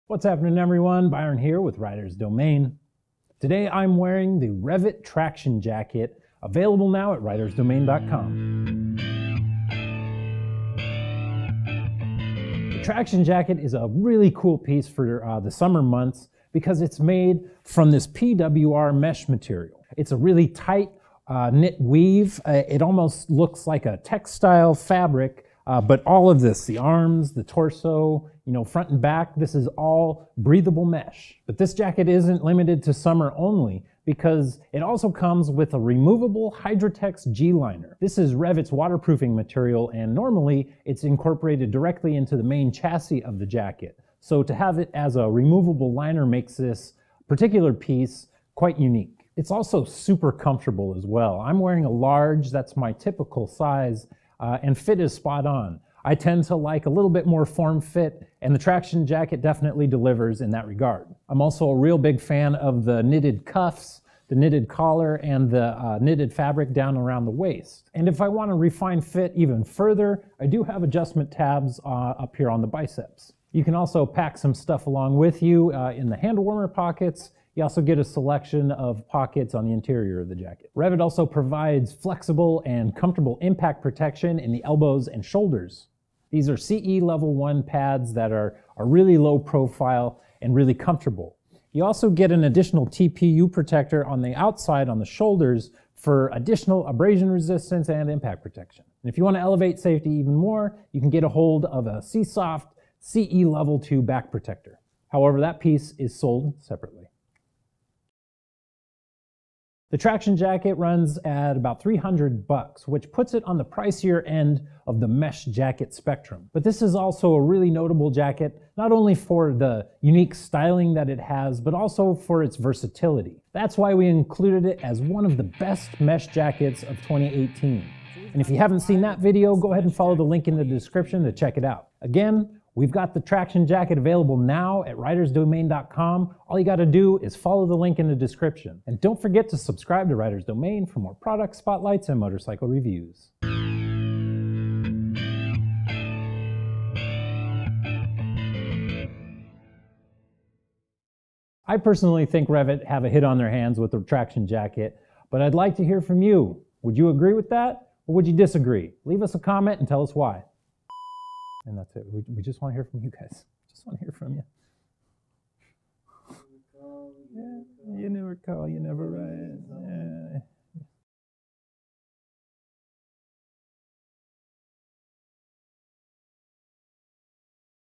REV'IT! Traction Jacket Spotlight Review | Riders Domain (2018)
Breathable and waterproof, the REV'IT! Traction jacket is a unique blend of style, functionality and protection.

The PWR | mesh chassis is light and well ventilated, while the removable hydratex | G-liner provides a much needed line of defense against rain.

As for impact protection, you get SEESMART armor in the elbows and shoulders, TPU sliders on the exterior of the shoulders and a pocket in the back that can hold a SEESOFT Level 2 back protector if you want to upgrade.

We liked the Traction jacket so much, that we included it in our Best Mesh Motorcycle Jackets of 2018. If you haven't seen that video yet, check it out

For more information on available colors and sizes, follow the link to RidersDomain.com in the description above.